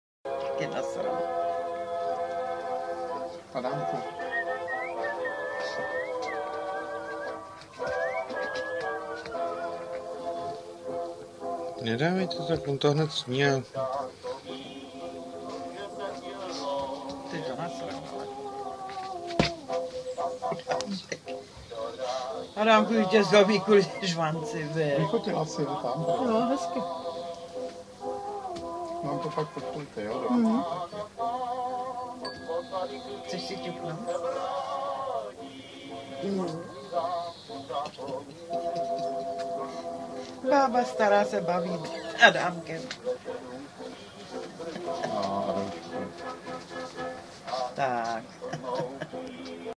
Slušně vychovaný pejsek
I pejsek muže o kousek jídla poprosit, a jednoho znám u mých rodičů v Mostě. - Autor: Karel  Procházka